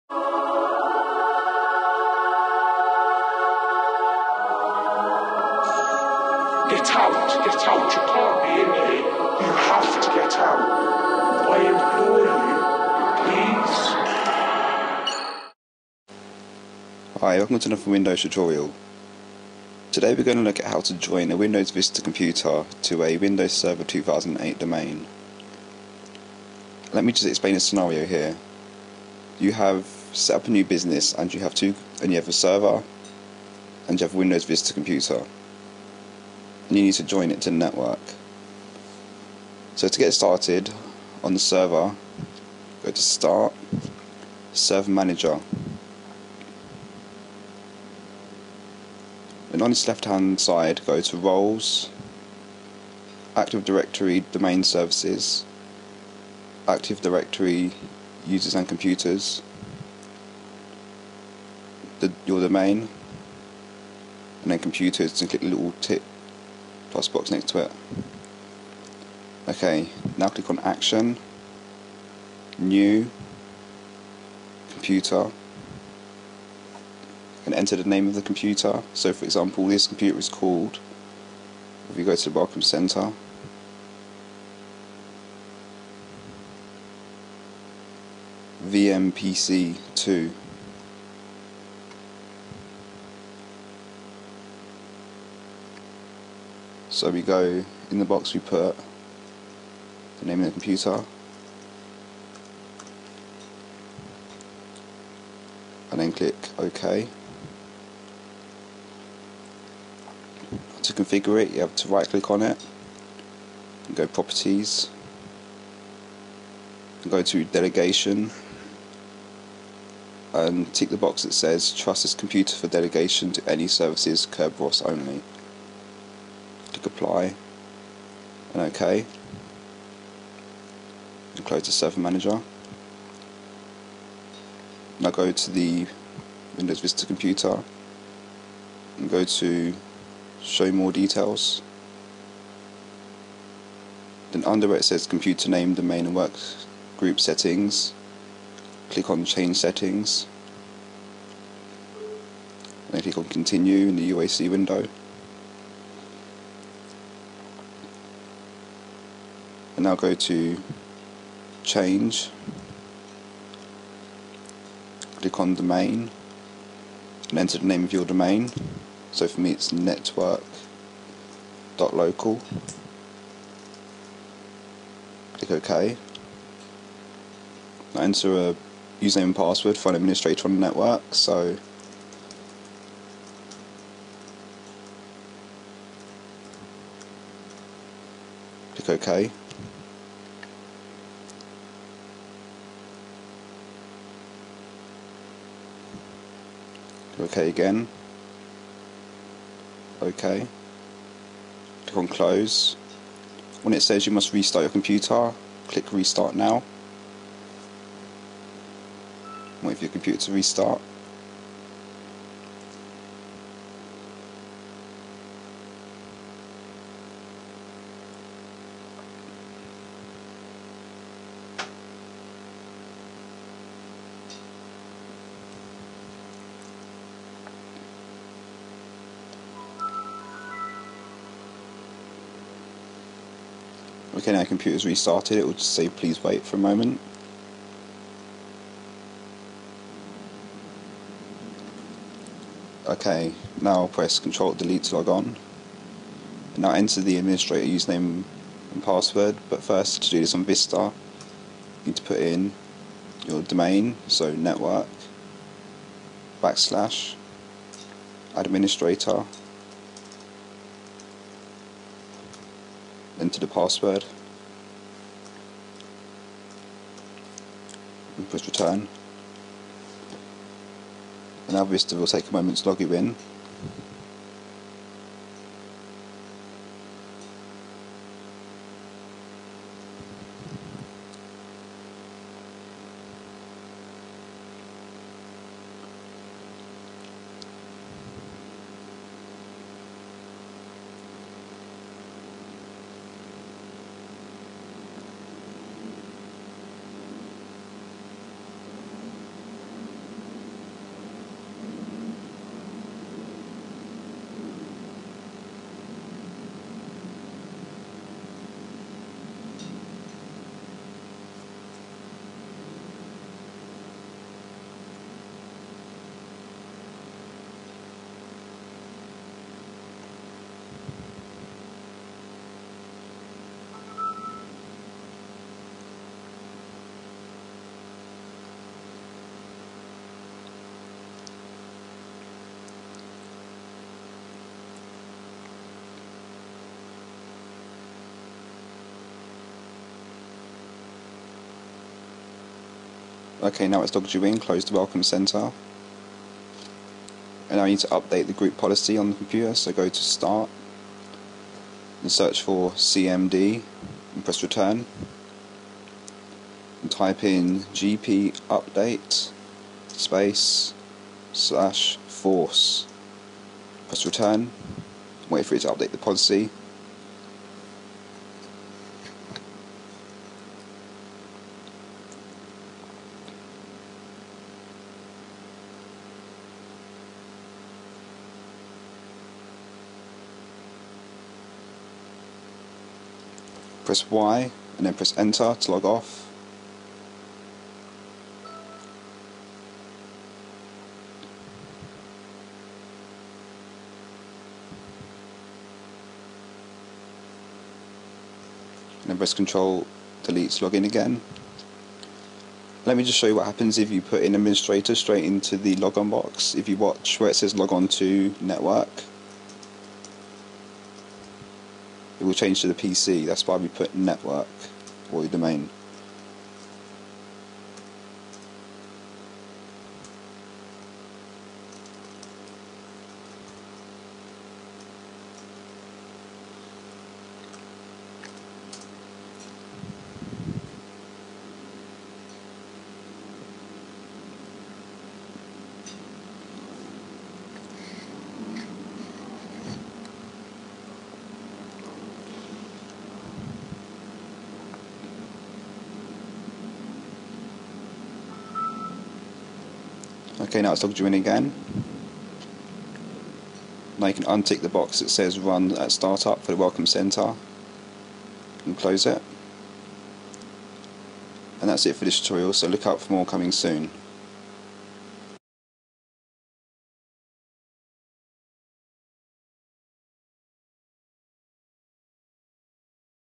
Joining a Windows Vista computer to a Windows Server 2008 domain - Windows Vista/Server 2008
This tutorial will show you how to join a computer running Windows Vista Business, Enterprise or Ultimate to a domain with a server running Windows Server 2008.